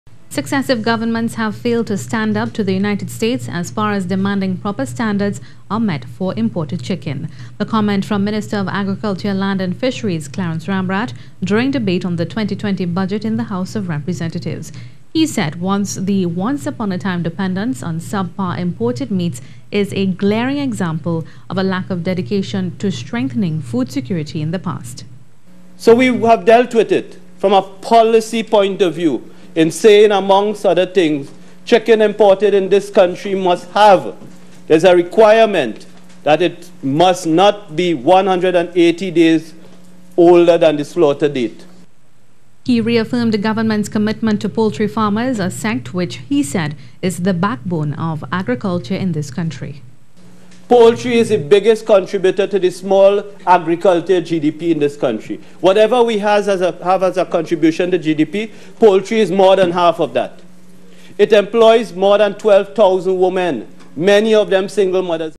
Better Standards For Imported Chicken
Successive Governments have failed to "stand up to the United States" as far as demanding proper standards are met for imported chicken.

The comment came from Minister of Agriculture, Land and Fisheries, Clarence Rambharat, during debate on the 2020 Budget in the House of Representatives.

He said the once-upon-a-time dependence on sub-par imported meats is a glaring example of a lack of dedication to strengthening food security in the past.

He reaffirmed Government's commitment to poultry farmers - a sect which he said is the backbone of agriculture in this country.